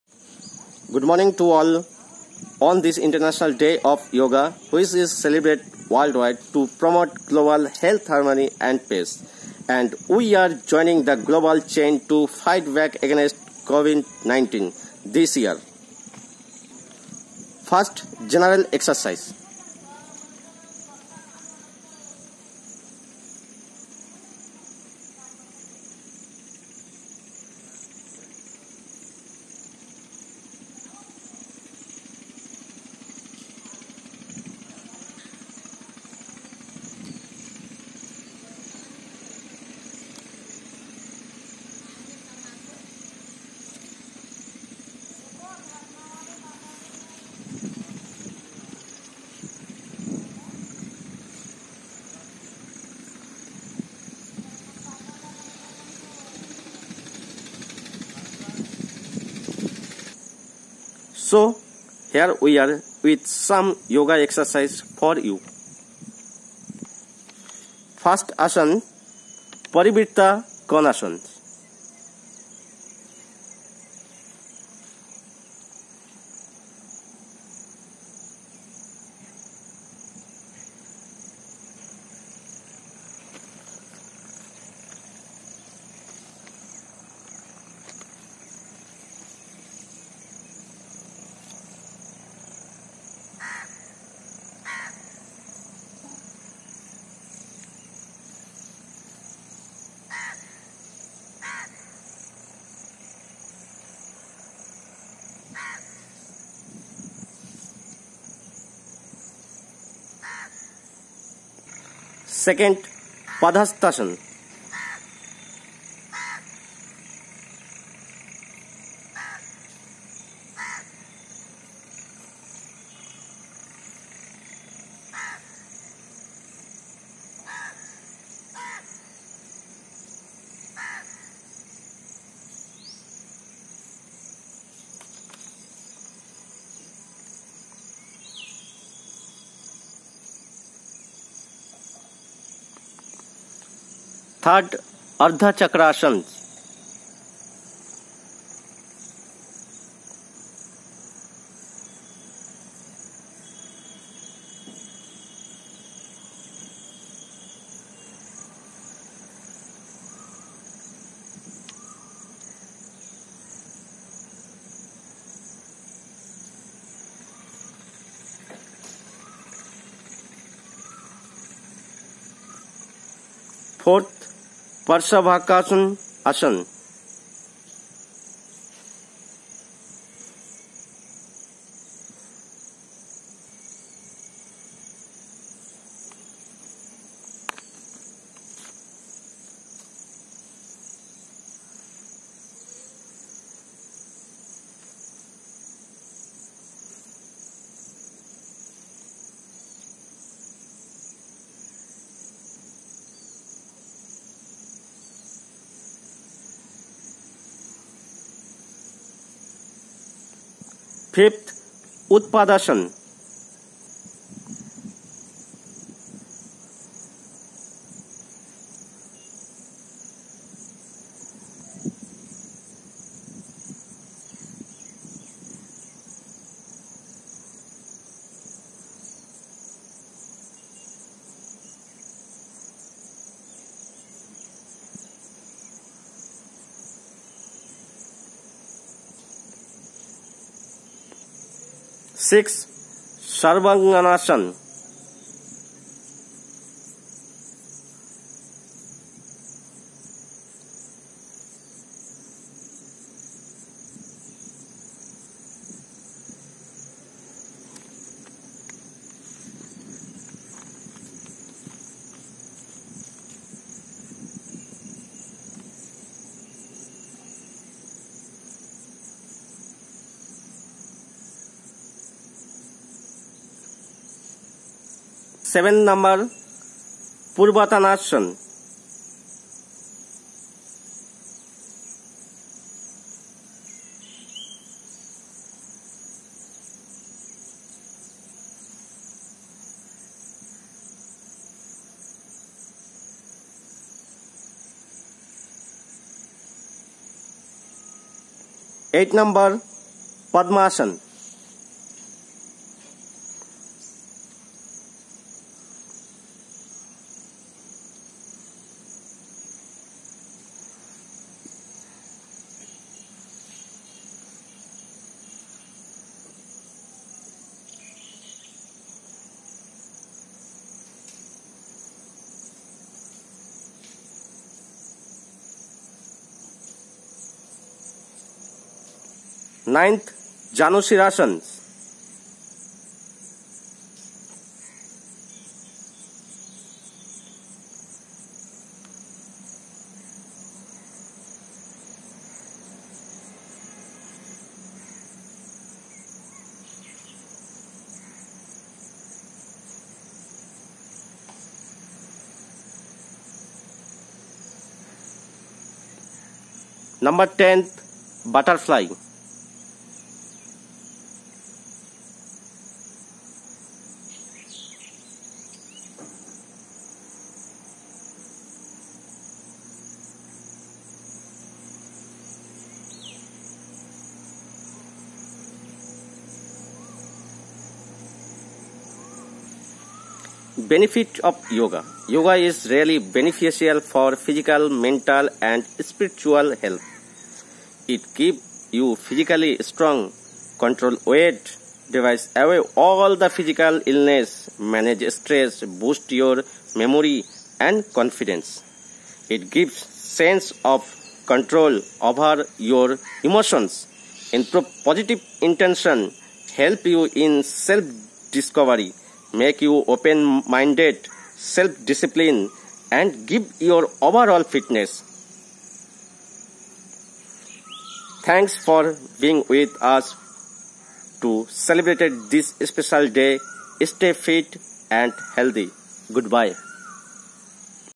**Celebration of International Yoga day at Gurukul Bhatapara**
This year the UN has given a special significance to Yoga and its practice with a view of making the nations of the world to become more aware of physical exercise &  to make it a form of home exercise. Yoga is a spiritual form of exercise originated in India around 6000 years ago. The observance of June 21st every year as International Yoga Day has brought a new colour and dimension to it. On this occasion of International Yoga Day, Gurukul school wishes you all year long happiness , wellness and safety.